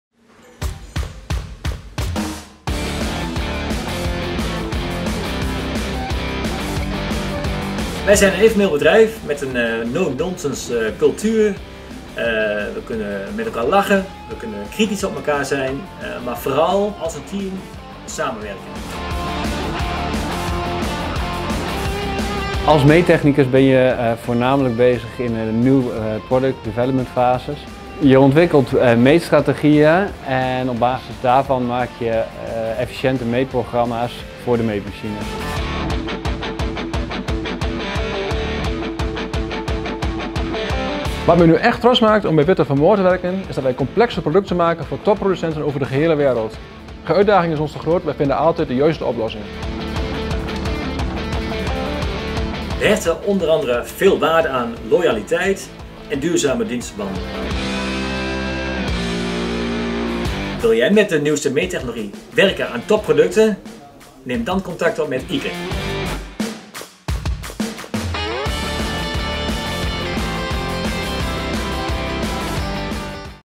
Meettechnicus bij Witte van Moort
Heb je ervaring met meettechniek en heb jij de ambitie om optisch meetspecialist te worden? Wil jij aan de slag gaan in een high-tech werkomgeving en heb jij oog voor detail? Dan is dit zeker interessant voor jou!

Als Meettechnicus bij Witte Van Moort ga jij je bezighouden met de Gom Scanbox, een hypermodern optische meetmachine. Je ontwikkelt, optimaliseert en beheert meetprogramma’s en strategieën. Verder geef je ook meetadvies tijdens het gehele productontwikkelingsproces.
Iets voor jou? Kijk op en neem contact op met Ike!